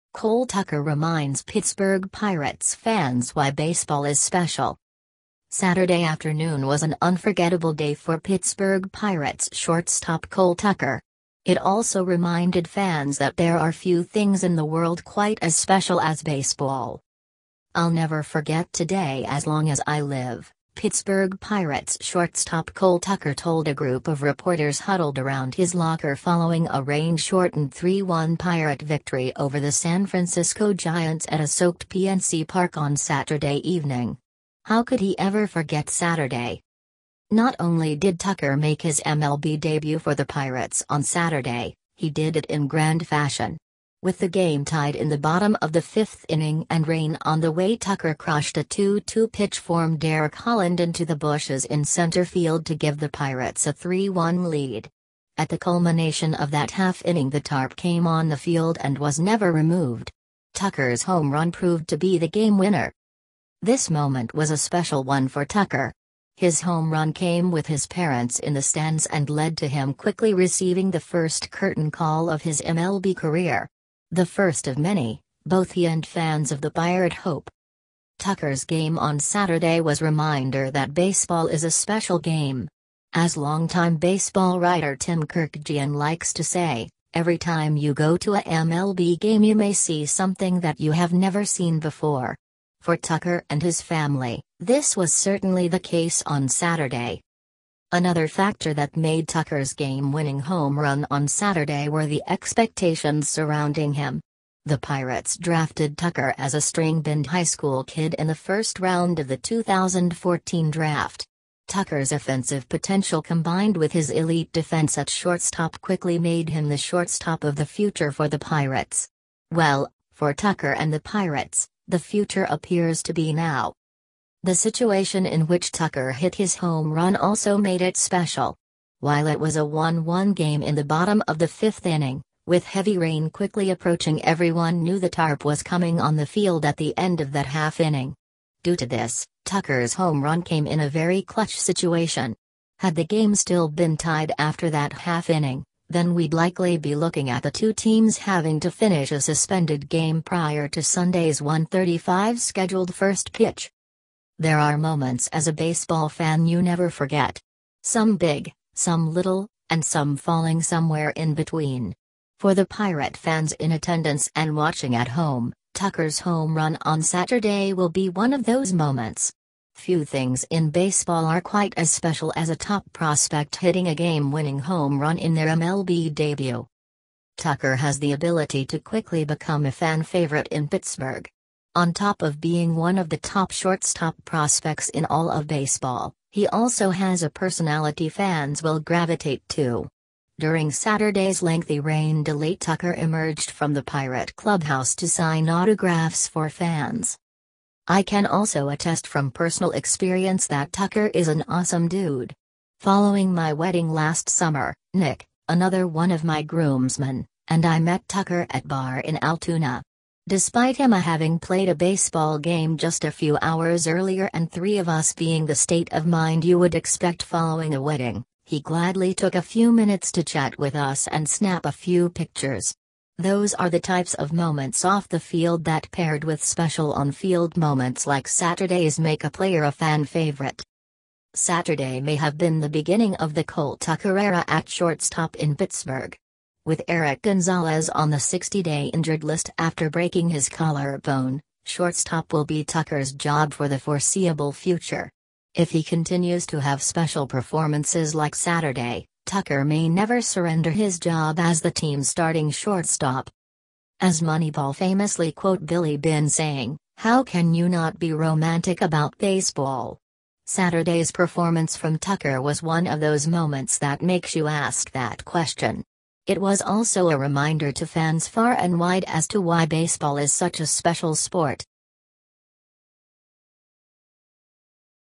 Cole Tucker Reminds Pittsburgh Pirates Fans Why Baseball Is Special
Saturday afternoon was an unforgettable day for Pittsburgh Pirates shortstop Cole Tucker. It also reminded fans that there are few things in the world quite as special as baseball.